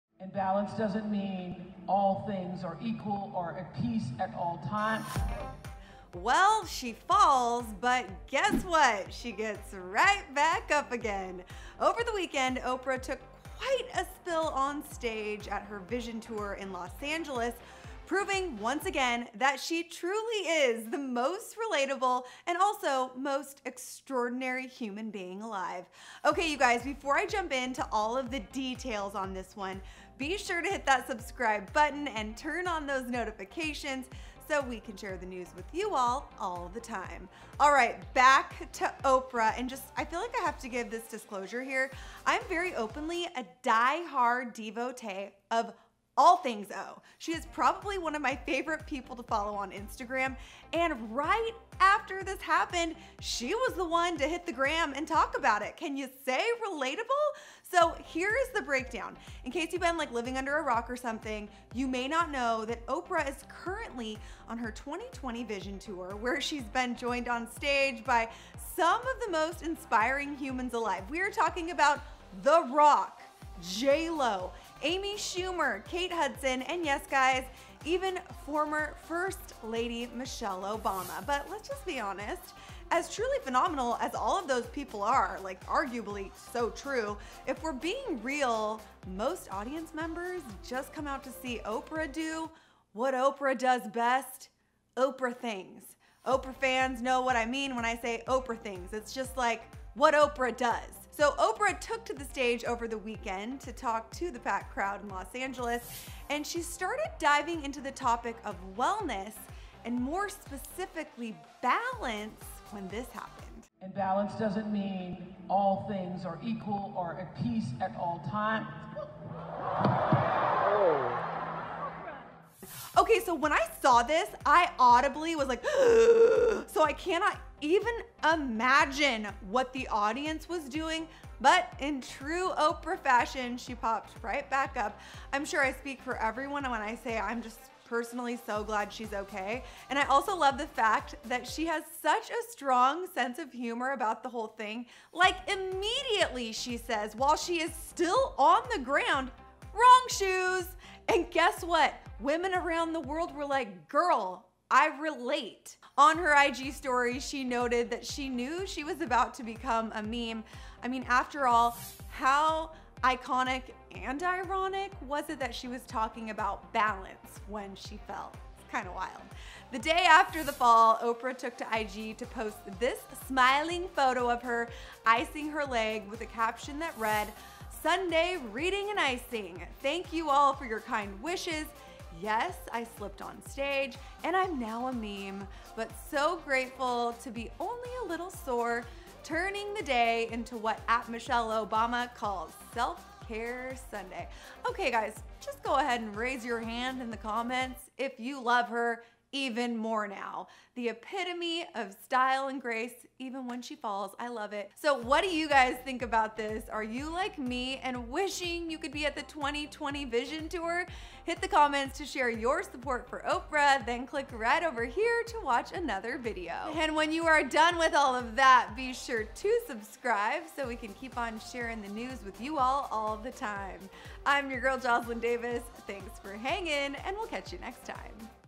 Oprah FALLS on Stage While Talking About Balance and Laughs it Off
She falls but guess what she gets right back up again! Over the weekend, Oprah took quite a spill onstage at her Vision tour in Los Angeles proving once again that she is truly the most relatable and also extraordinary human being alive.

Ok guys before I jump in to all the details on this one, be sure to hit

Alright back to Oprah – and full disclosure here, I’m very openly a
die-hard devotee of all things O. She is probably my favorite person to follow on Instagram and right after this happened she was the one to hit the gram and talk about it. Can you say relatable?

SO here’s the breakdown, in case you’re living under a rock, you may not know that Oprah is currently on her 2020 Vision tour where she’s been joined on stage by some of the most inspiring humans alive – we’re talking The Rock, JLo, Amy Schumer, Kate Hudson and yes guys, even former First Lady Michelle Obama.

But let’s be honest, as truly phenomenal as these guests are, if we’re being real, most audience members come out to see Oprah do what she does best – Oprah things. Oprah fans know what I mean when I say Oprah things.

Oprah took to the stage over the weekend to talk to the packed crowd in Los Angeles and started diving into the topic of wellness and more specifically balance, when this happened:

When I saw this I audibly gasped so I can’t even imagine what the audience was doing. But in true Oprah fashion, she popped right back up. I’m sure I speak for everyone when I say I’m so glad she’s ok.